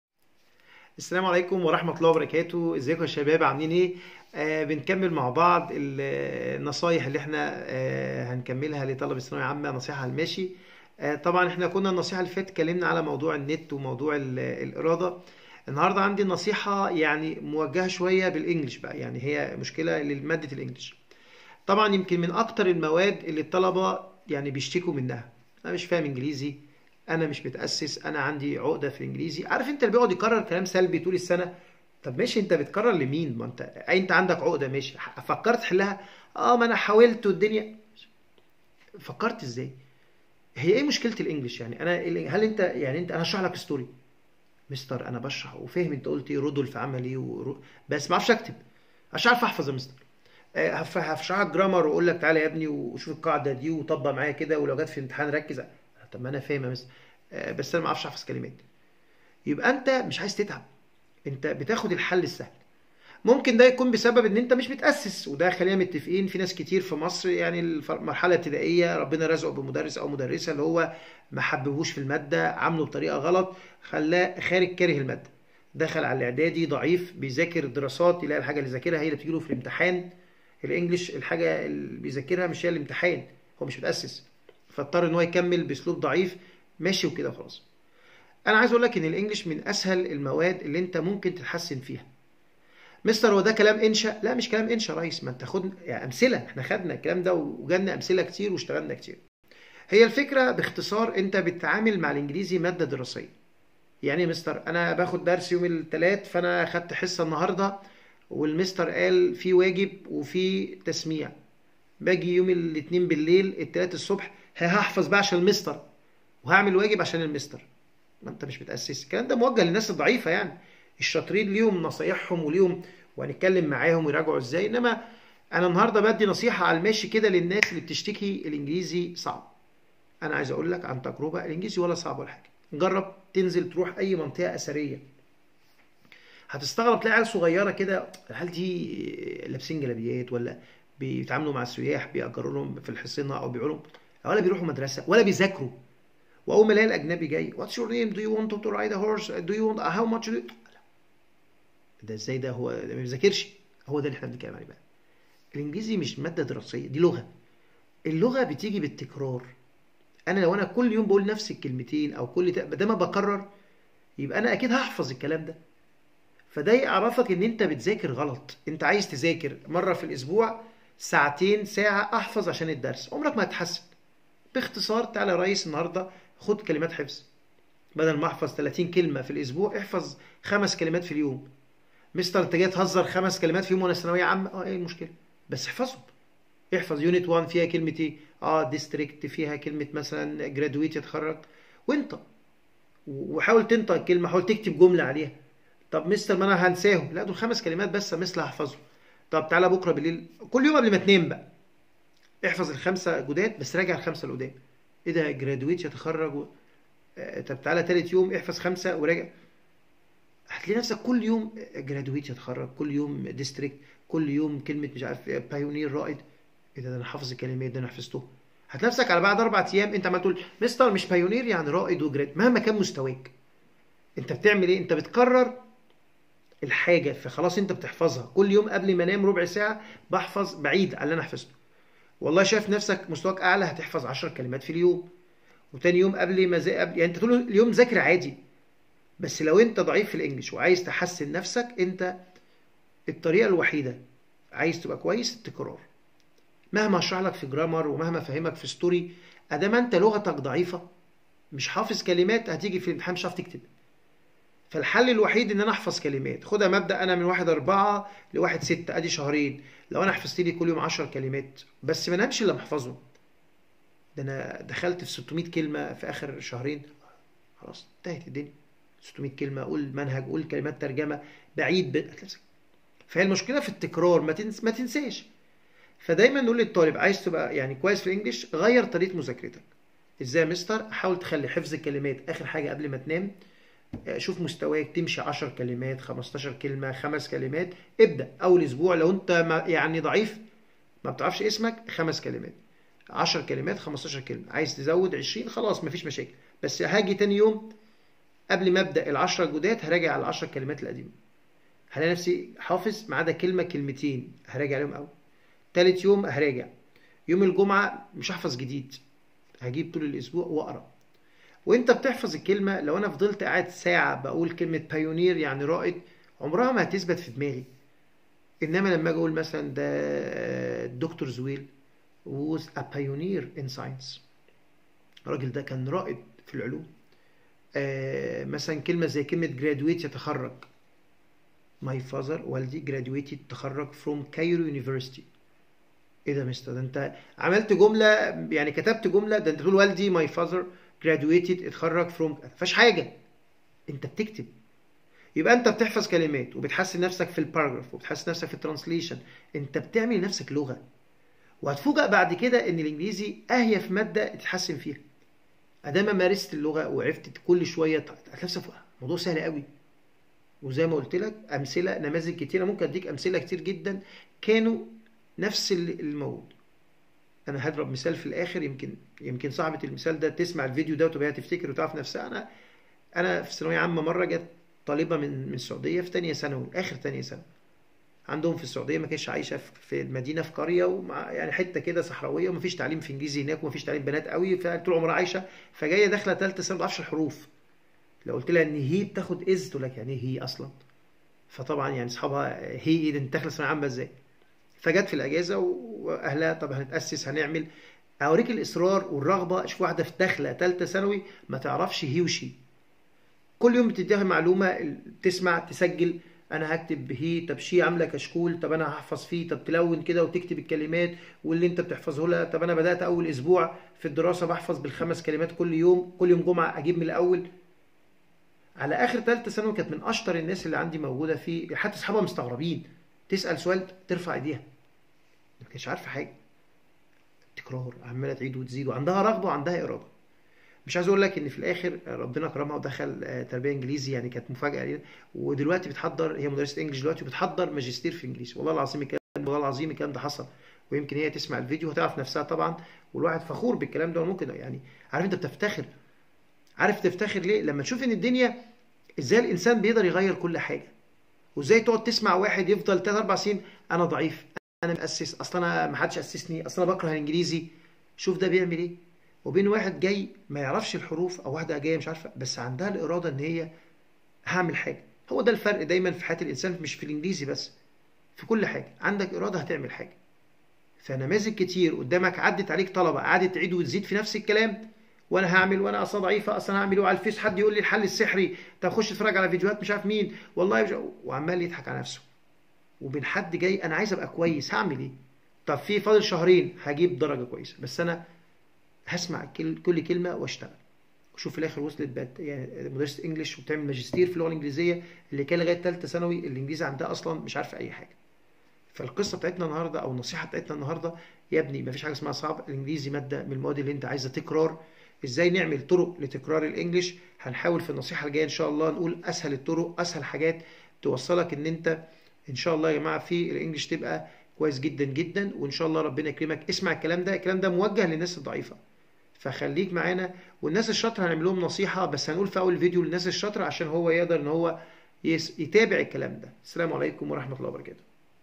ازاي نحل عقدة الانجليزي ( نصيحة على الماشي 2)
يقدم مستر محمد الديب لابناءه طلاب الثانوية العامة سلسلة حلقات نصيحة على الماشي لحل أهم المشاكل إللي بتواجه الطلبة. شير في الخير لكل اصحابك علشان كله يستفيد من الفيديو.